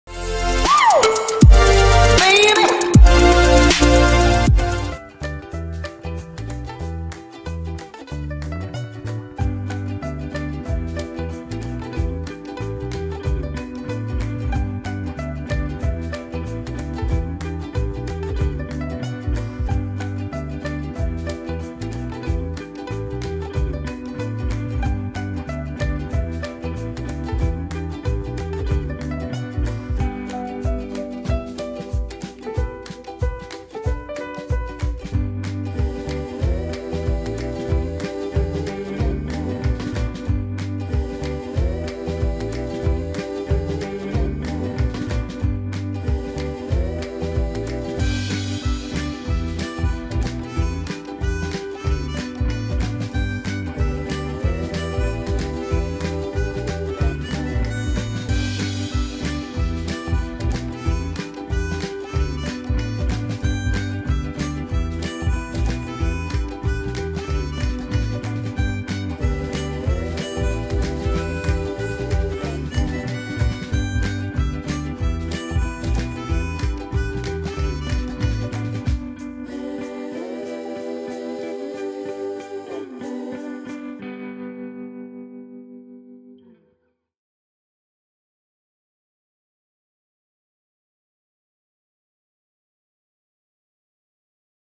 2014 Audi S1 - Monday 01/03/21
This stunning Audi S1 goes under the hammer on the 01/03/21. In striking yellow you certainly won’t be missed on the road! At 227bhp this pocket rocket will certainly be fun!!